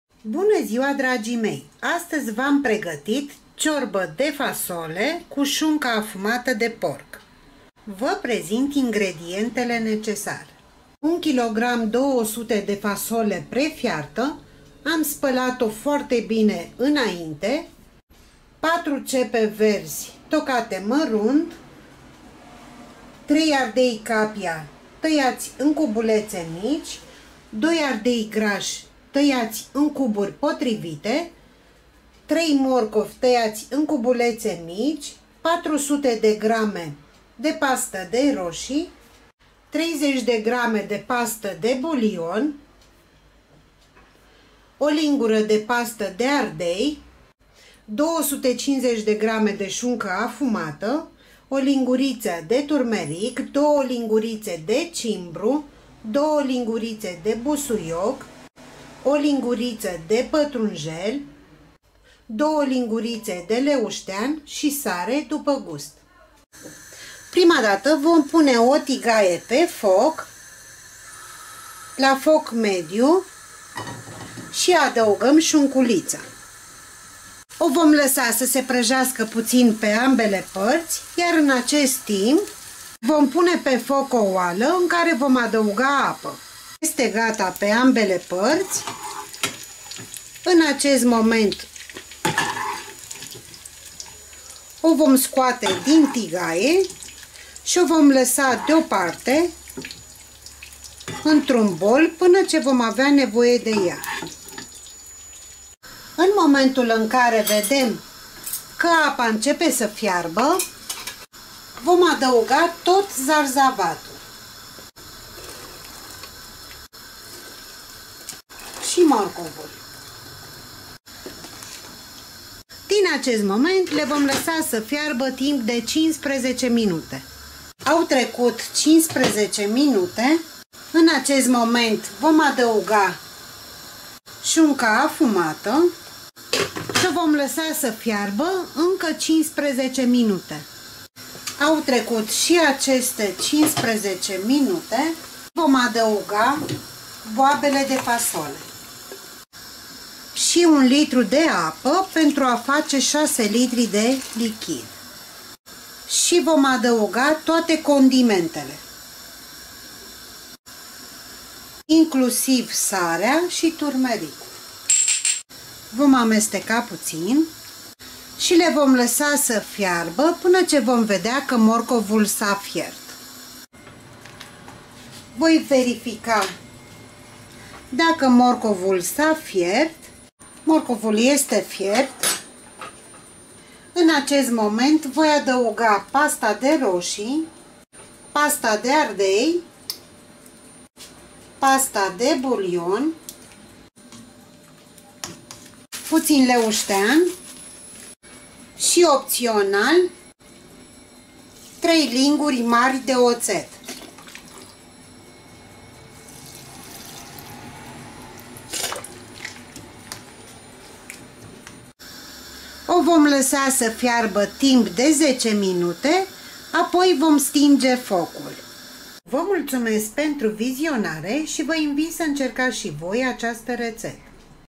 Ciorba de Fasole cu Afumatura sau Ciorba de Fasole Boabe cu Sunca de Porc Retete de Ciorba de Fasole
O Reteta de Ciorba de Fasole foarte usor de facut si super gustoasa ce se poate servi in orice moment al zilei.
Încercați si voi aceasta Reteta de Ciorba de Fasole cu Sunca Afumata de Porc si va asigur ca o sa va încânte.

🔴👇👇👇Pentru a nu pierde nici o rețetă, vă puteți abona cu un click pe acest link 👇👇👇🔴